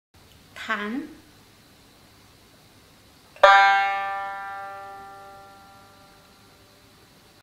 中国 三弦 奏法 「弾」
中国 三弦 奏法 「弾」｜Playing techniqu,‘tan’, sanxian, China
費堅蓉（三弦）｜Fei Qianrong (sanxian) （Rec. 2003 | Tokyo, Japan）
許可なく転載またはダウンロードすることを固く禁じます。